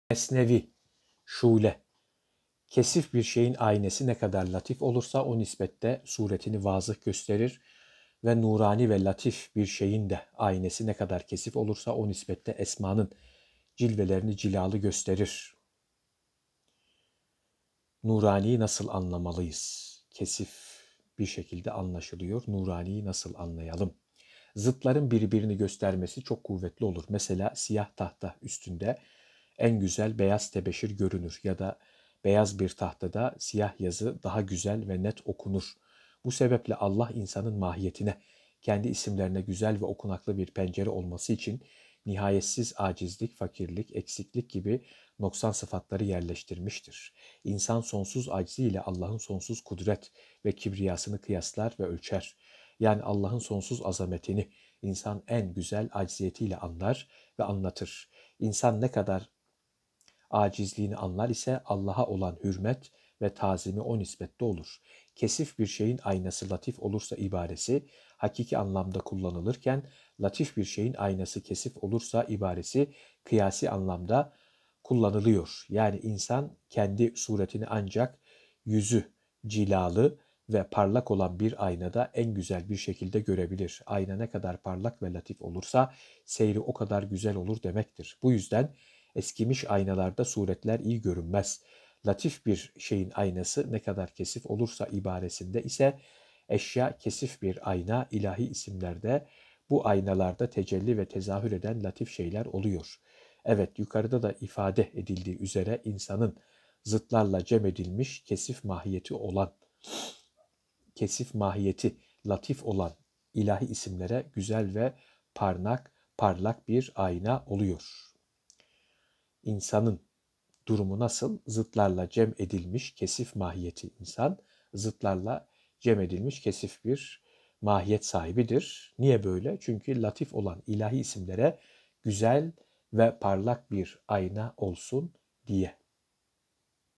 Allah'ın isimlerinin insanın mahiyet aynasında yansıması?
Mesnevi-i Nuriye Şule
Kesif bir şeyin âyinesi ne kadar latif olursa, o nisbette suretini vâzıh gösterir. Ve nuranî ve latif bir şeyin de âyinesi ne kadar kesif olursa, o nisbette esmanın cilvelerini cilâlı gösterir.